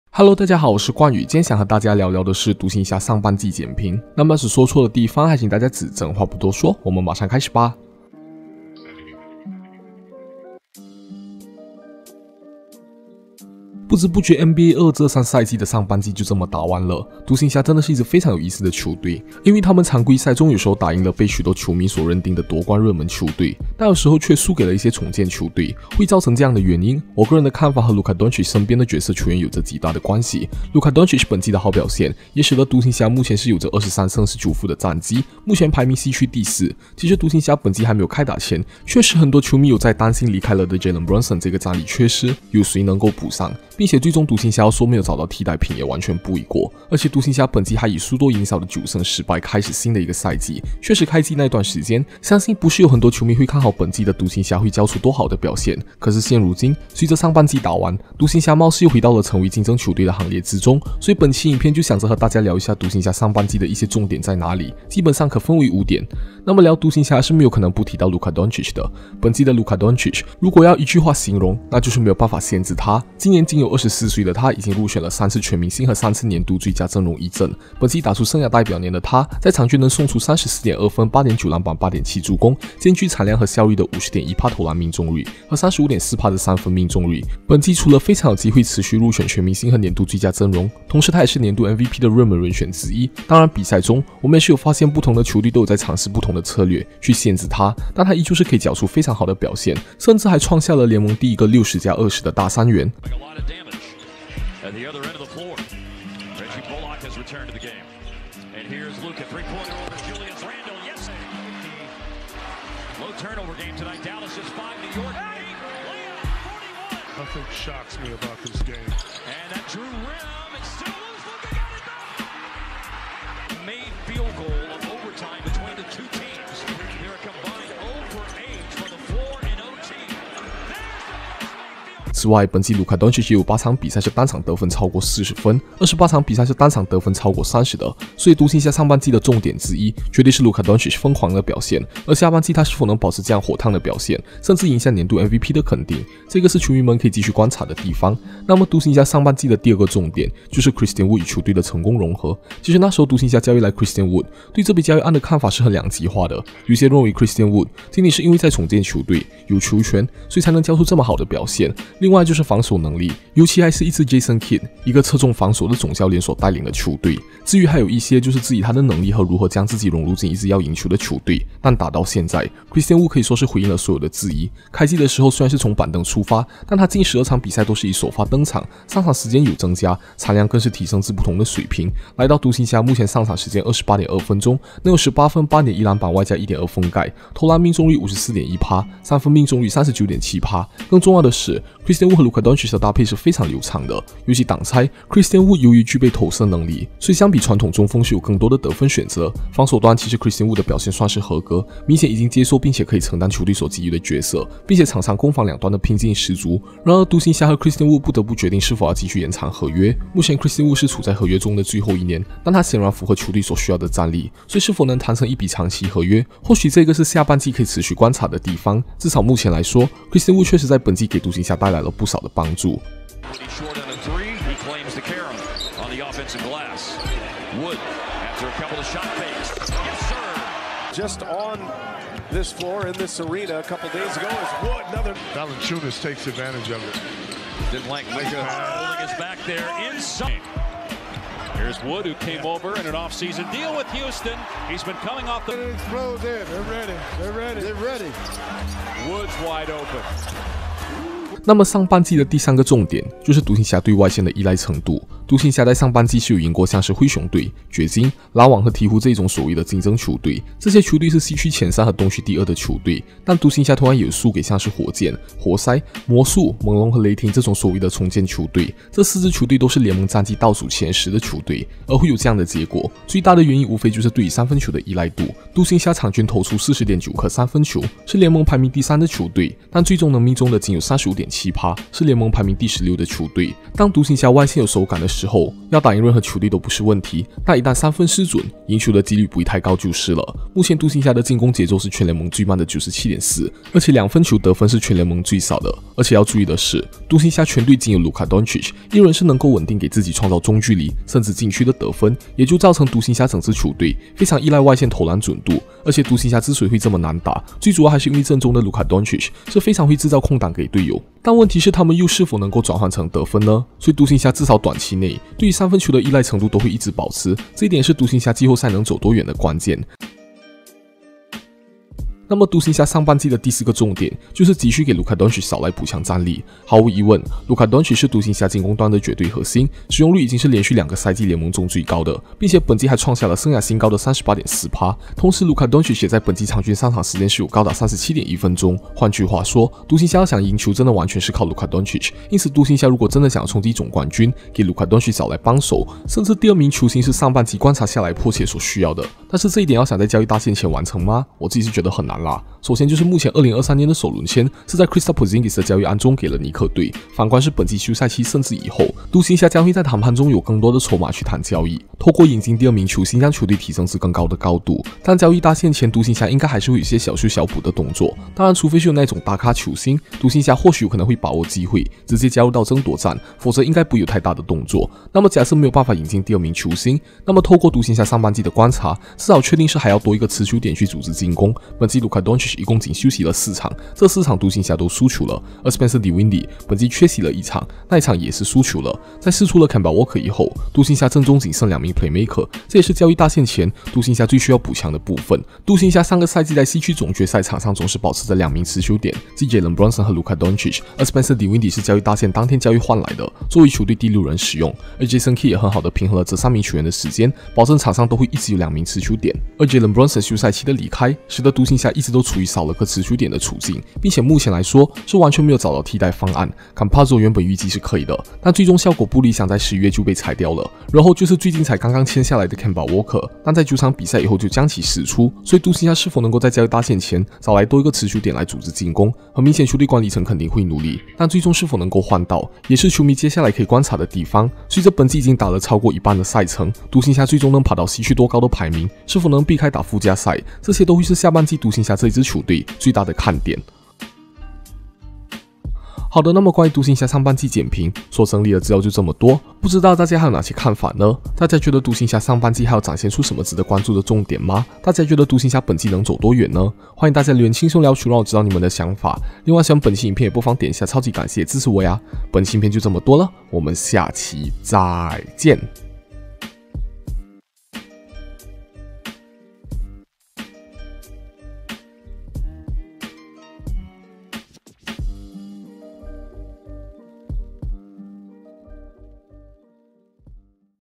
NBA 🏀 五個重點總結，獨行俠上半季簡評！獨行俠還有哪些不足？本季獨行俠能走多遠？（冠祤聊球）
不知不覺NBA 22-23賽季的上半季就這麼打完了。獨行俠真的是一支非常有意思的球隊，因為他們常規賽中有時候打贏了被許多球迷所認定的奪冠熱門球隊，但有時候卻輸給了一些重建球隊。會造成這樣的原因，我個人的看法和Luka Doncic 身邊的角色球員有著極大的關係。 Luka Doncic 本季的好表現，也使得獨行俠目前是有著23勝19負的戰績，目前西區排名第四。其實獨行俠本季還沒有開打前，確實很多球迷有在擔心離開了的Jalen Brunson這個戰力缺失，有誰能夠補上？並且最終獨行俠要說沒有找到替代品也完全不為過。而且獨行俠本季還以輸多贏少的9勝10敗開始新的一個賽季。確實開季那段時間，相信不是有很多球迷會看好本季的獨行俠會交出多好的表現。可是現如今，隨著上半季打完，獨行俠貌似又回到了成為競爭球隊的行列之中。所以本期影片，就想著和大家聊一下獨行俠上半季的一些主要重點在哪裡，基本上可分為五點。